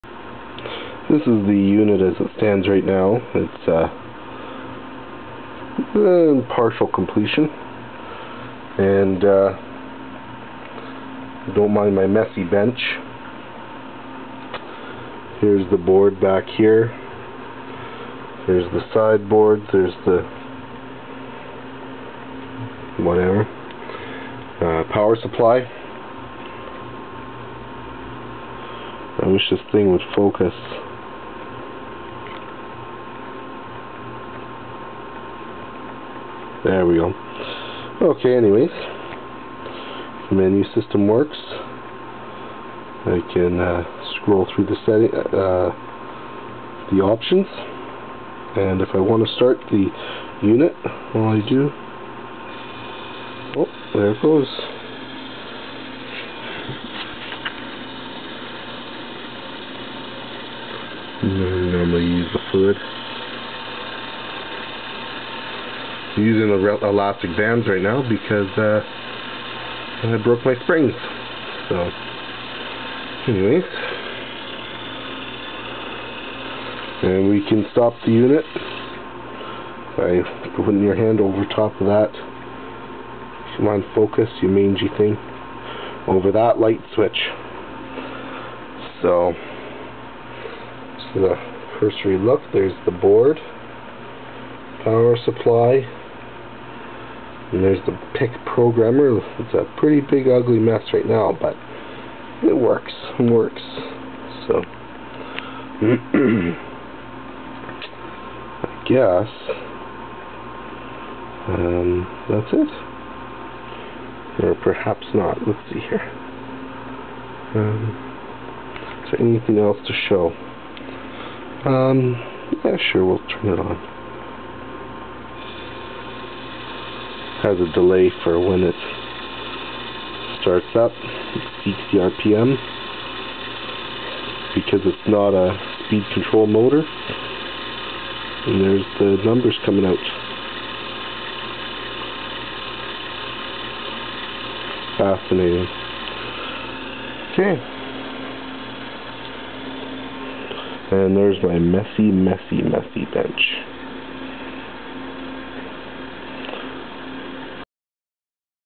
My Stormer Viscometer - a la eleventy billion
This is a video of an almost DIY stormer like viscometer. This one operated a little differently that most in that many things are easily adjustable and could be used for linear viscosity measurements. It's been a while in the making but it's been fun! You can see some of my other work

As you can see it still needs some work...